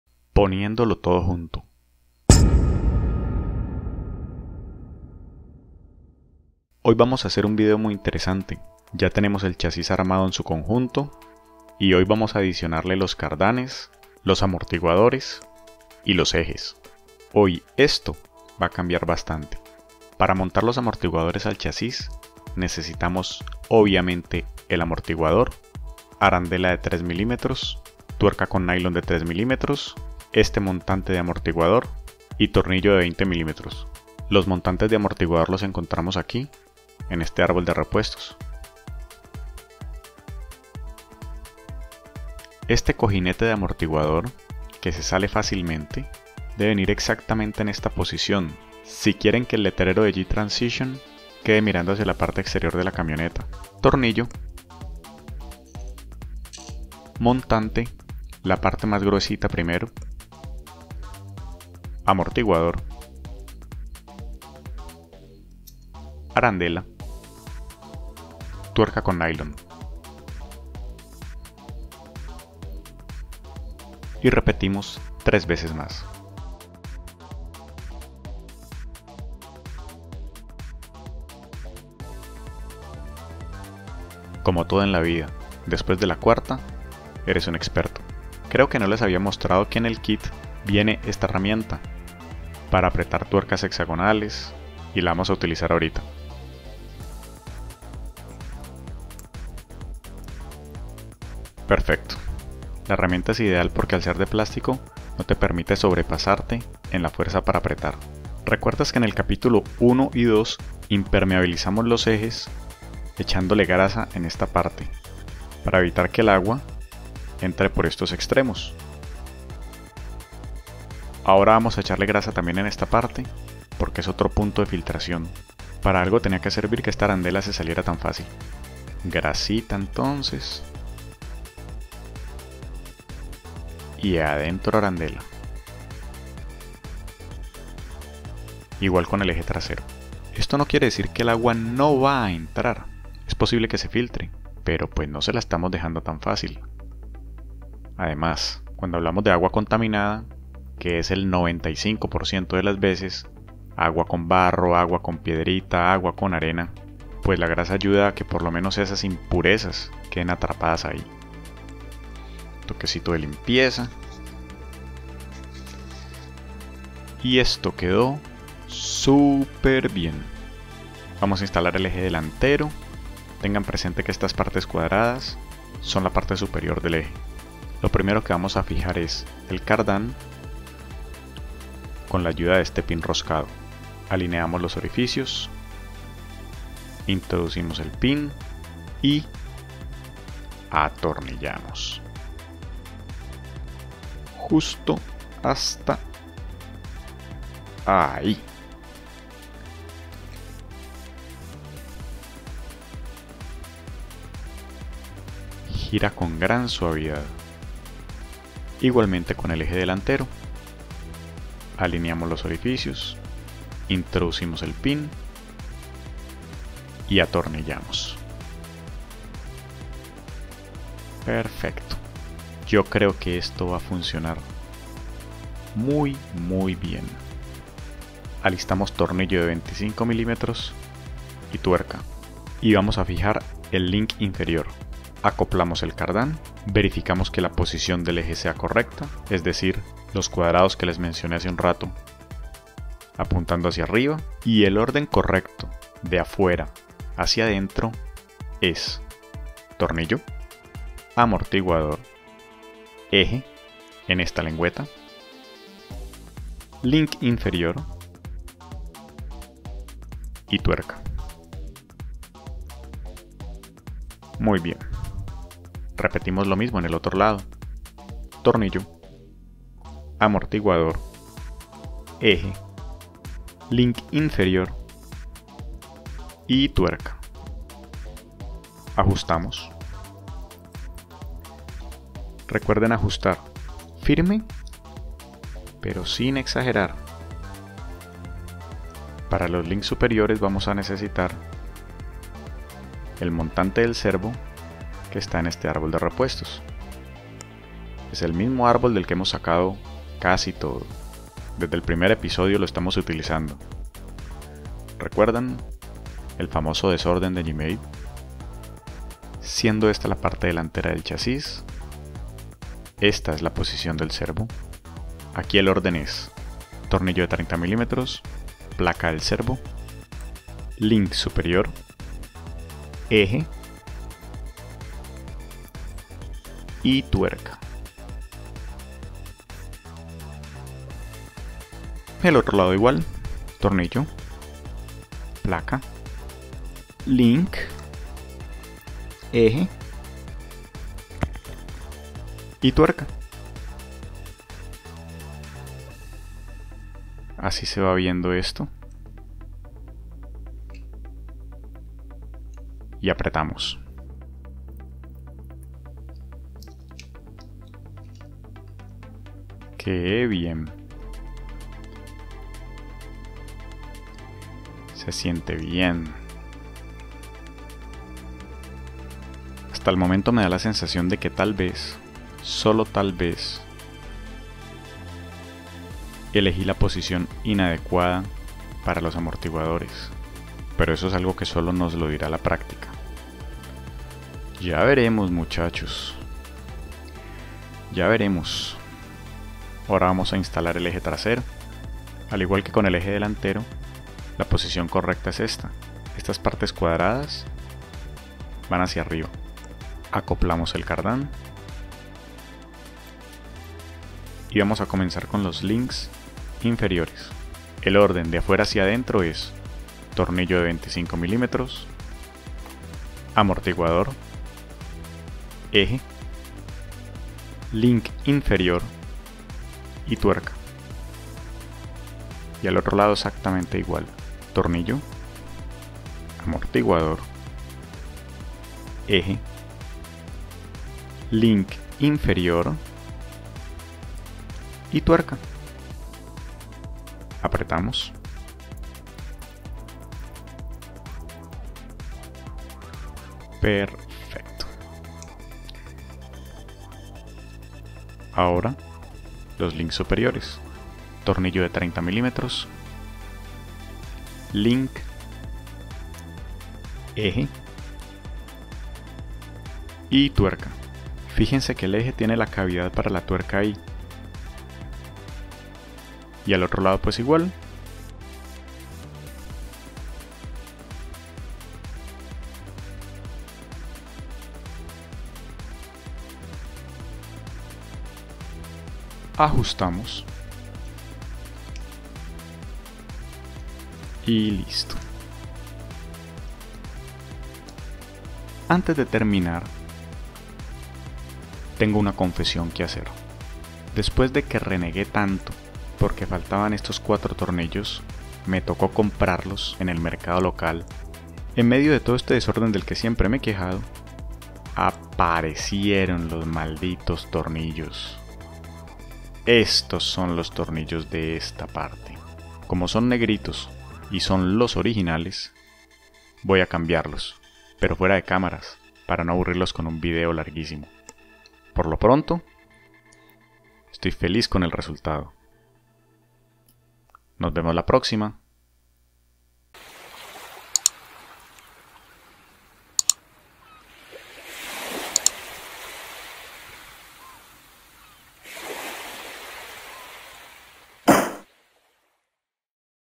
GS01 Komodo Build Series Ep.11 – Putting It All Together – Espanol
Assembling the axles, links and universal shafts to the chassis.
Ensamblando los ejes, los links y los cardanes al chasis.

If you’re not building this exact same truck, I’ll assure you this series going to serve you anyway.
My new project:
Build the GS01 Gmade Komodo Kit from out of the box to the first run. I’ll explain in detail the whole process, step by step and with a little sense of humor, you know me by now.

Main unit:
Gmade Komodo GS01 Scale 1.9 Crawler Kit
Duratrax Onyx NiMH 6C 7.2V 3000mAh (with dean plug attached)

Upgrades:
RadioLink RC3S 2.4G 4 Channel Transmitter TX
R4EH-G 2.4G 4CH Receiver For Radiolink RC3S RC4G Transmitter
Axial 55T Rock Crawler Electric Motor
Axial AE-5 Brushed Waterproof ESC w/Reverse Star Plug
Tactic TSX45 Standard High-Torque Metal Gear 2BB Servo
Everything else is stock.

Please let me know any comments and suggestions.
I’m sorry for my little English. I’m a native spanish speaker.

P.D. If interested, I myself shoot with a Samsung Galaxy S6, a Samsung Galaxy S3 and a Samsung Galaxy J7 Prime. Everything in HD 1080p.